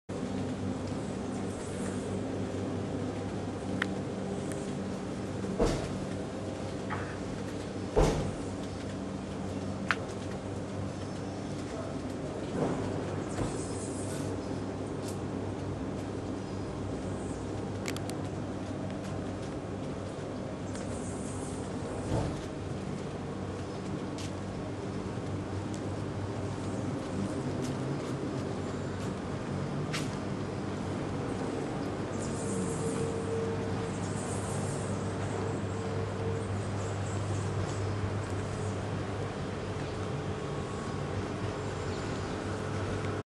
Barbados Almond Beach
A walk through the complex..toward the Sugar Mill..where you can get married..